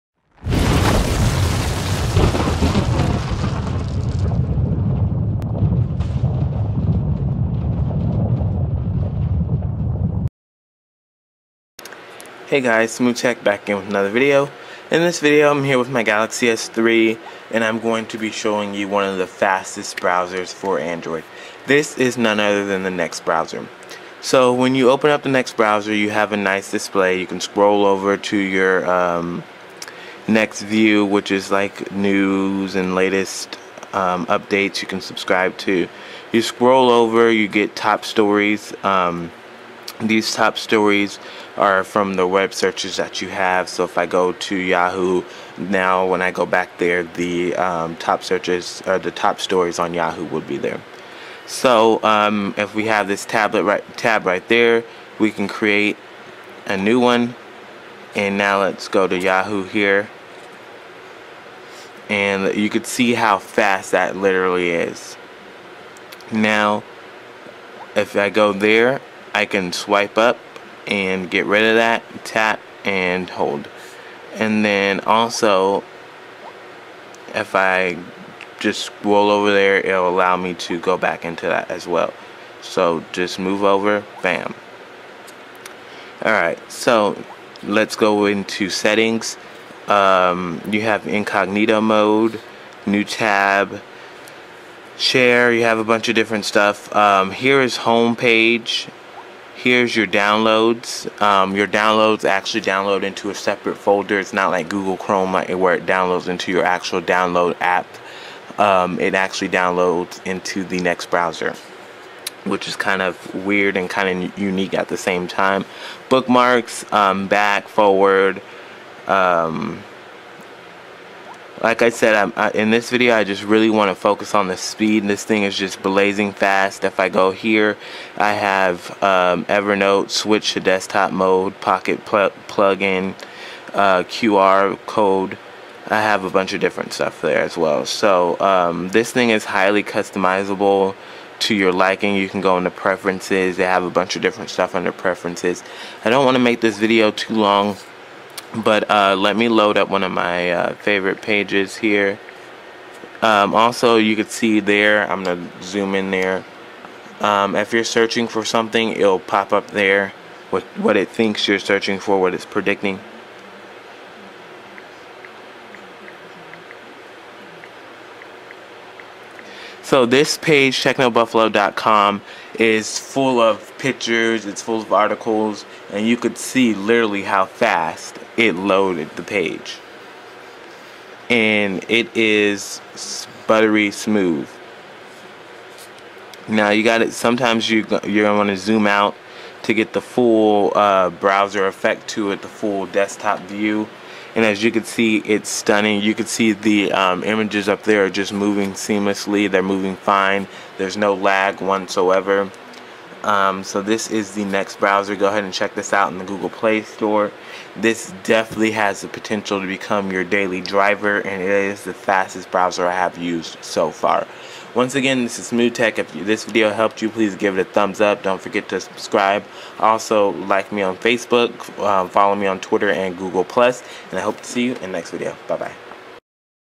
App Review: " Fastest Android Browser"
This will be a review of the fastest Android browser that i have ever used to date thus far.